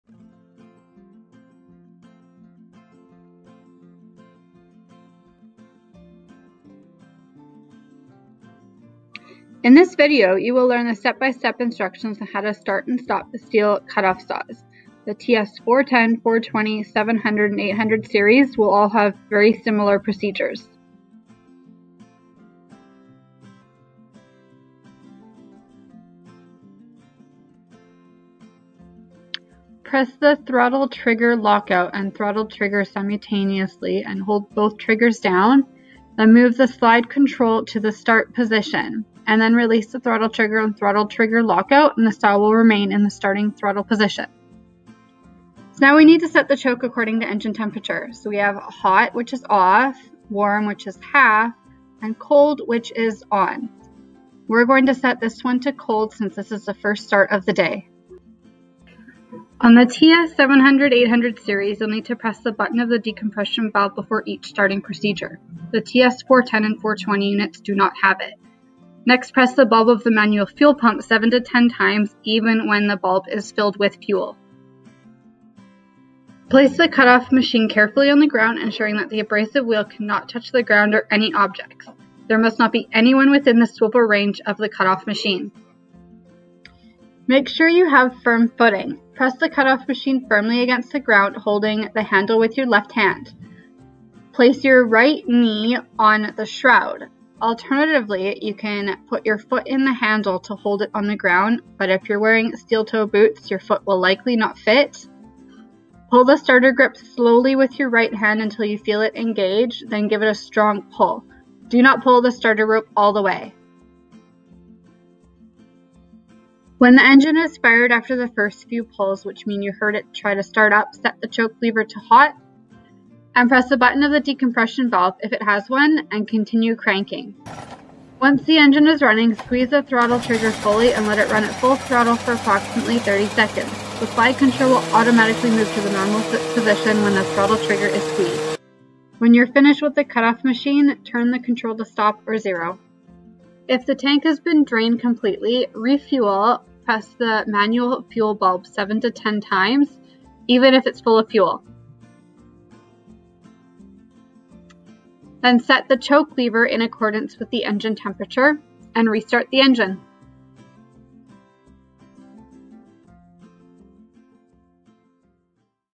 "How-To" Series: Starting and stopping a cutoff saw (AKA Chop saw)
Step by step instructions to properly and safely start and stop your cut off saw.

When using, always consider personal protective equipment such as safety glasses and gloves.